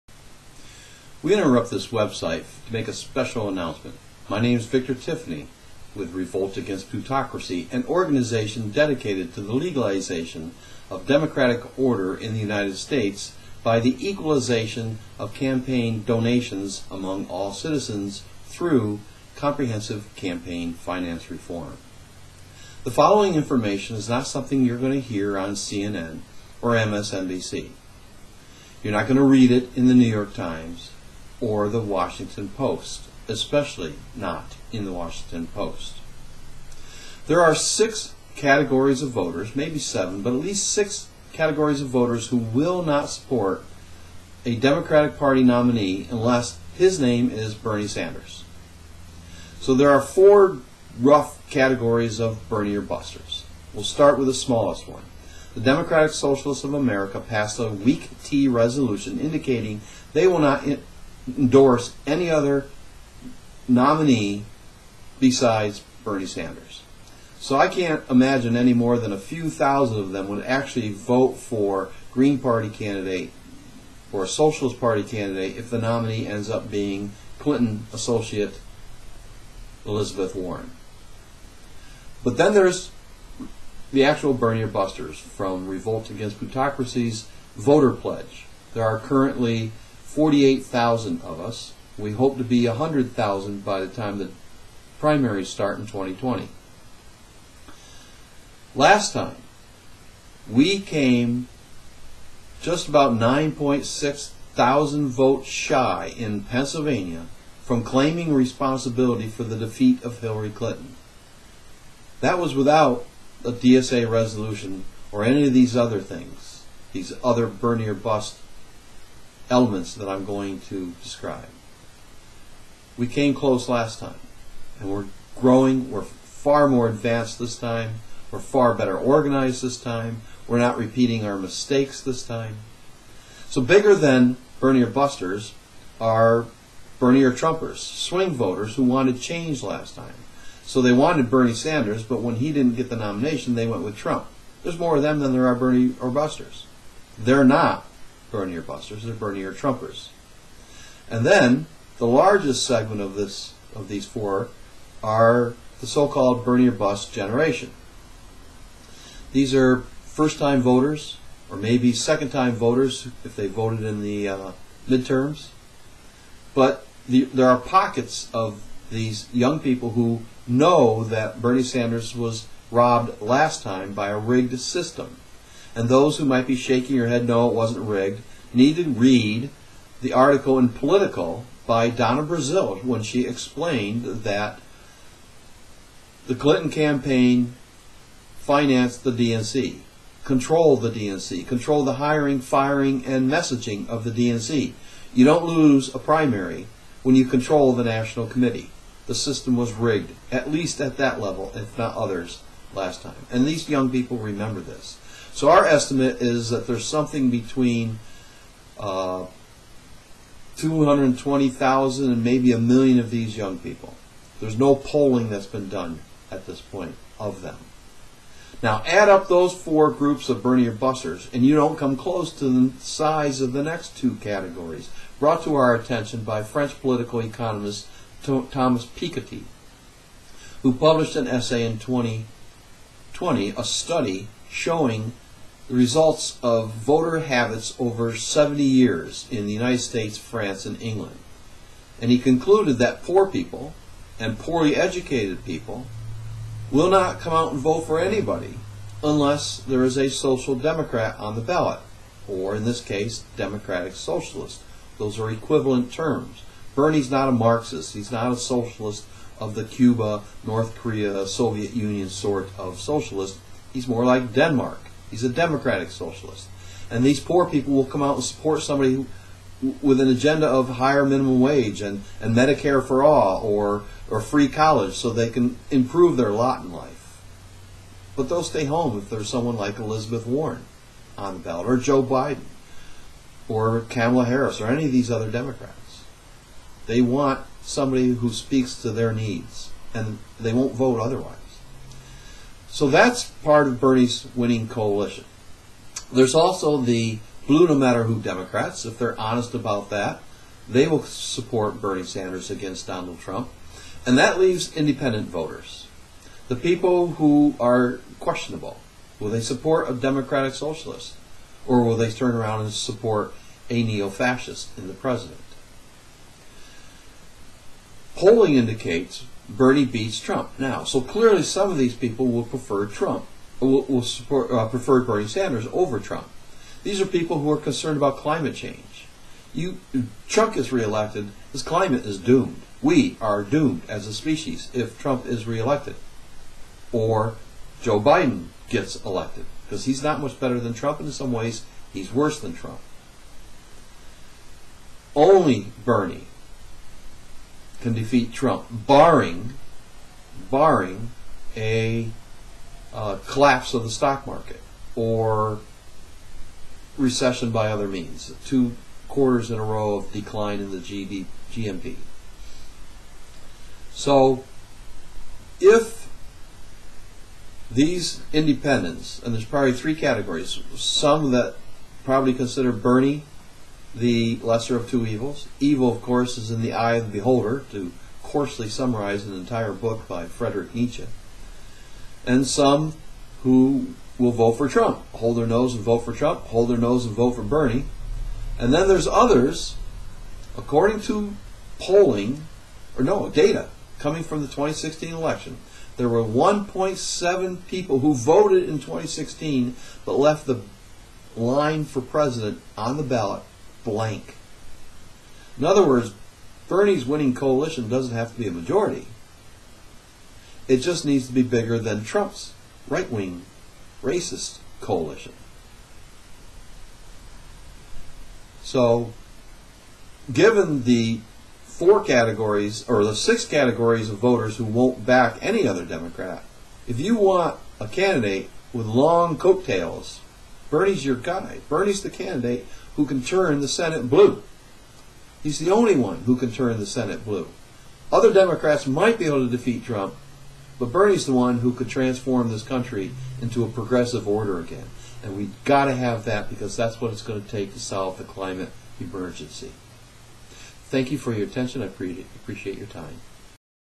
Special Announcement
Victor explains why Bernie has the best, if not the only shot of defeating Trump in 2020.